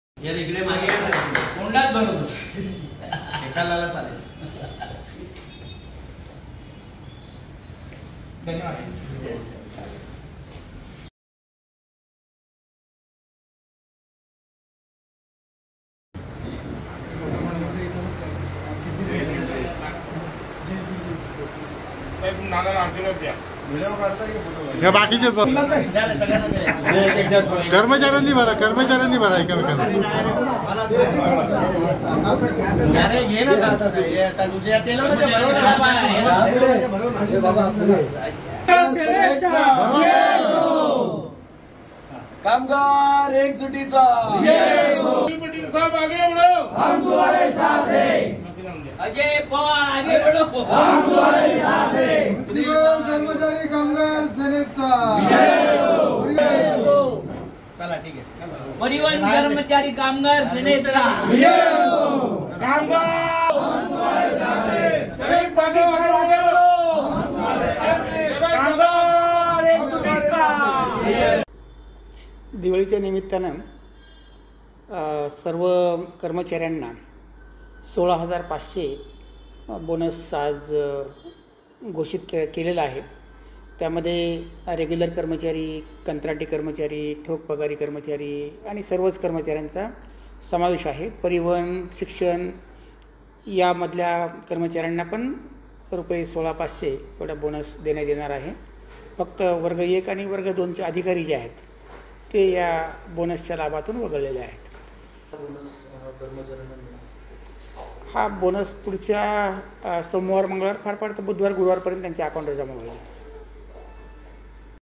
KALYAN | महापालिका कामगारांना मिळणार 16 हजार 500 रुपये दिवाळी बोनस
कल्याण डोंबिवली महापालिका कामगारांना 16 हजार 500 रुपये बोनस जाहिर करण्यात आला आहे. ही घोषणा महापालिका आयुक्त डॉ. भाऊसाहेब दांगडे यांनी दिली आहे मागच्या वर्षी 15 हजार 500 रुपये बोनस दिला गेला होता. यंदा त्यात एक हजार रुपयांची वाढ करण्यात आली आहे. यंदा 16 हजार 500 रुपये बोनस जाहिर करण्यात आला आहे. बोनसची रक्कम येत्या बुधवार्पयत कामगारांच्या खात्यात जमा होईल असे आयुक्तांनी सांगितले. रेग्यूलर, ठोकपगारी, कंत्राटी, परिवहन आणि शिक्षण खात्यातील कामगारांना हा बोनस दिला जाणार आहे. केवळ वर्ग एक आणि दोनच्या अधिकार्याना बोनस दिला जाणार नाही हे आयुक्तांनी स्पष्ट केले आहे. बोनस जाहिर होताच कामगारांनी आनंद व्यक्त करीत एकमेकांना पेढे भरविले.

BYTE - डॉ. भाऊसाहेब दांगडे, आयुक्त, केडीएमसी.